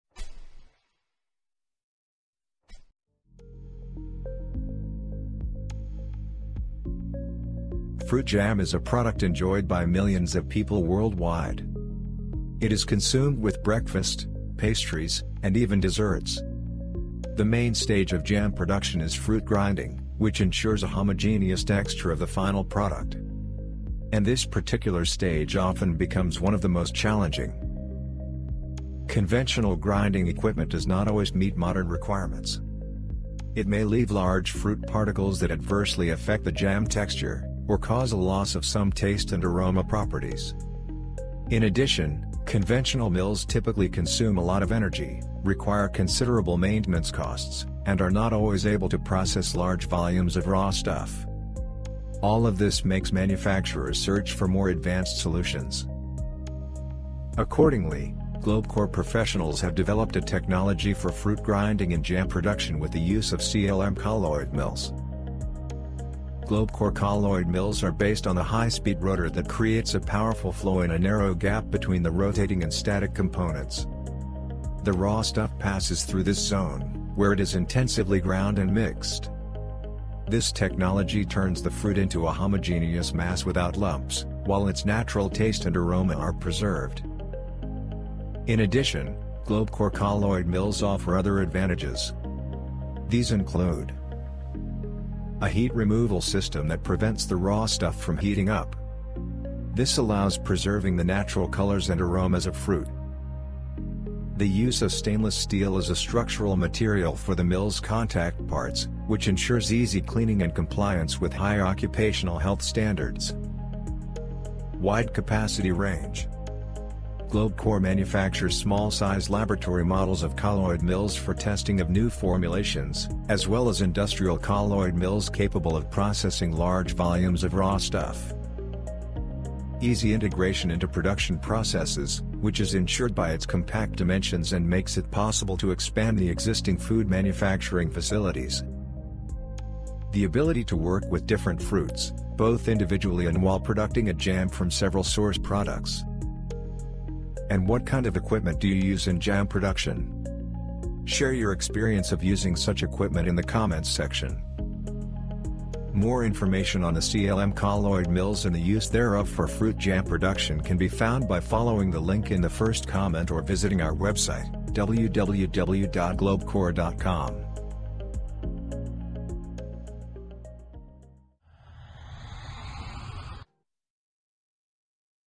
Fruit Jam Production With The Use Of CLM Colloid Mills
More details We present to your attention jam production technology and equipment CLM.

Fruit jam is a product enjoyed by millions of people worldwide. It is consumed with breakfast, pastries, and even desserts. The main stage of jam production is fruit grinding, which ensures a homogeneous texture of the final product. And this particular stage often becomes one of the most challenging.

Conventional grinding equipment does not always meet modern requirements. It may leave large fruit particles that adversely affect the jam texture, or cause a loss of some taste and aroma properties. In addition, conventional mills typically consume a lot of energy, require considerable maintenance costs, and are not always able to process large volumes of raw stuff. All of this makes manufacturers search for more advanced solutions.

Accordingly, GlobeCore professionals have developed a technology for fruit grinding in jam production with the use of CLM colloid mills.

GlobeCore colloid mills are based on a high-speed rotor that creates a powerful flow in a narrow gap between the rotating and static components. The raw stuff passes through this zone, where it is intensively ground and mixed. This technology turns the fruit into a homogeneous mass without lumps, while its natural taste and aroma are preserved.

In addition, GlobeCore colloid mills offer other advantages. These include:

● a heat removal system that prevents the raw stuff from heating up. This allows preserving the natural colors and aromas of fruit;
● the use of stainless steel as a structural material for the mill’s contact parts, which ensures easy cleaning and compliance with high occupational health standards;
● wide capacity range — GlobeCore manufactures small-sized laboratory models of colloid mills for testing of new formulations, as well as industrial colloid mills capable of processing large volumes of raw stuff;
● easy integration into production processes, which is ensured by its compact dimensions and makes it possible to expand the existing food-manufacturing facilities;
● the ability to work with different fruits, both individually and while producting a jam from several source products.